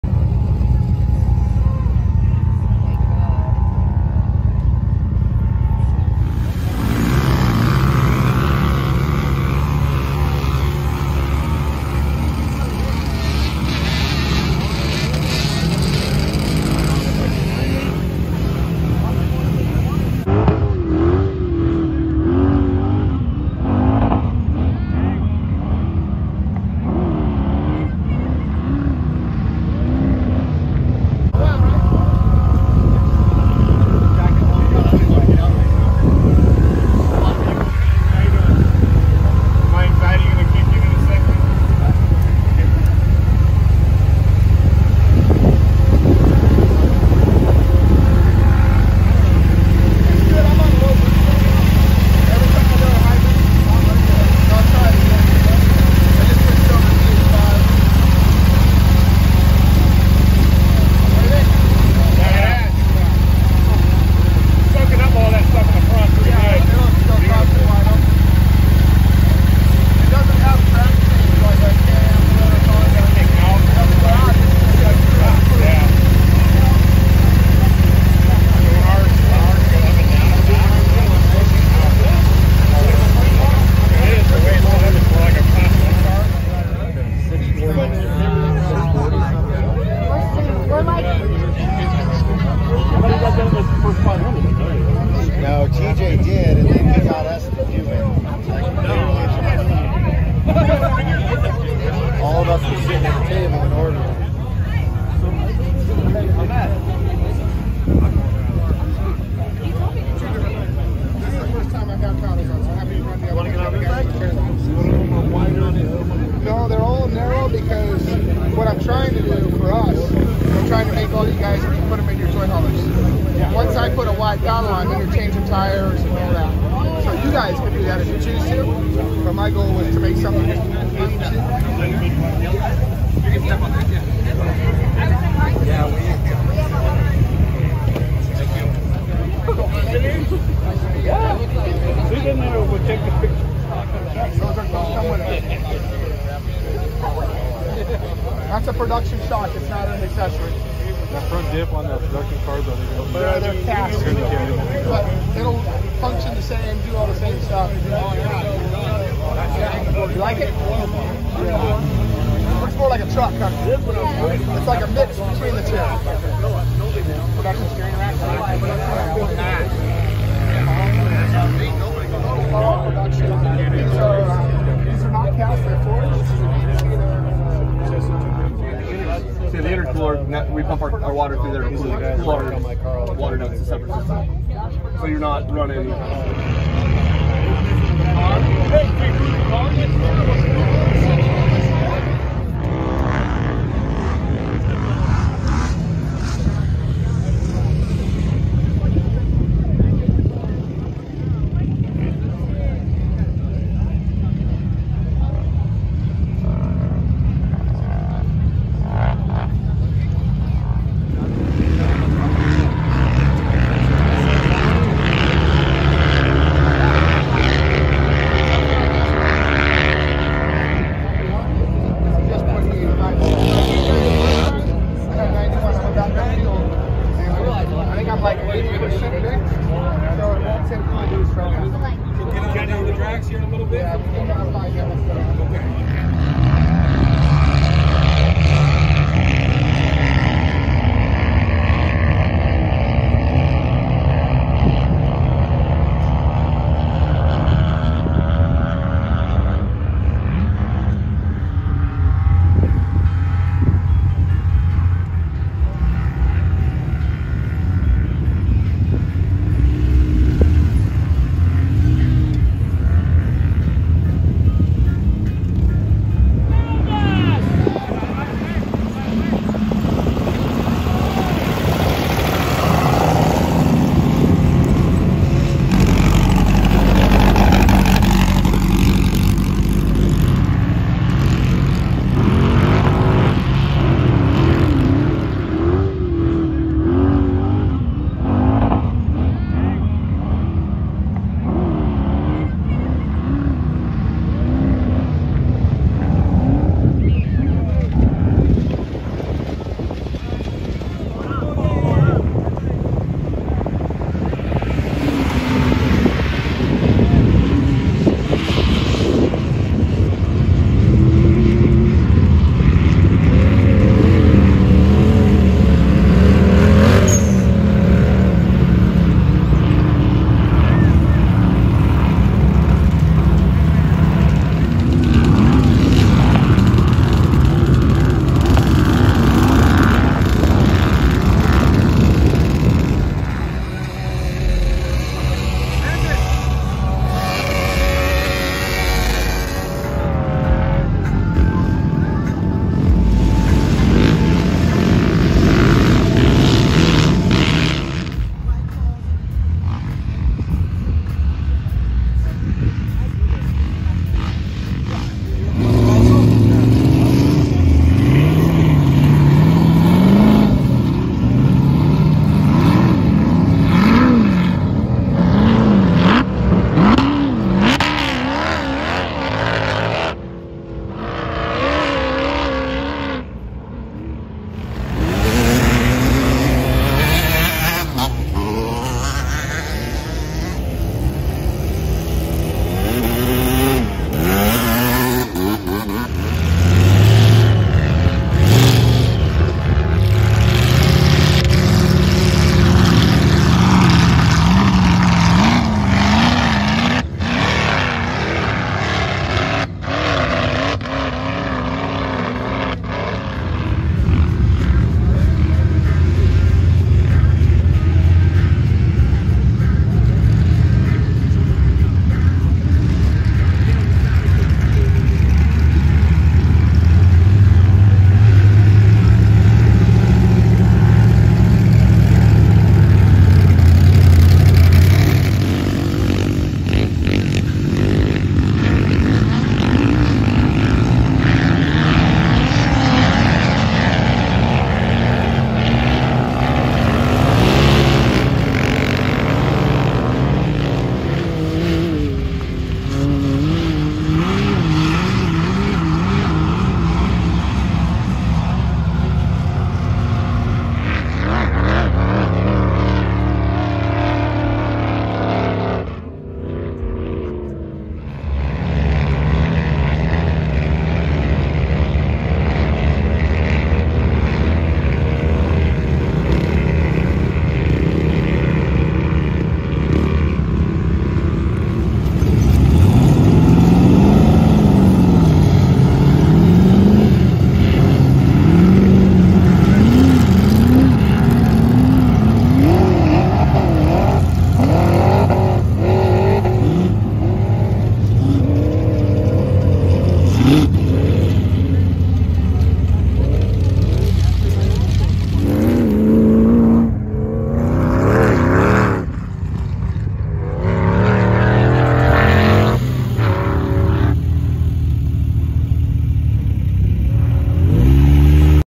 New Robby Gordon 300hp UTV Glamis Oldsmobile Hill
Get to meet Robby Gordon Saturday Glamis Oldsmobile hill.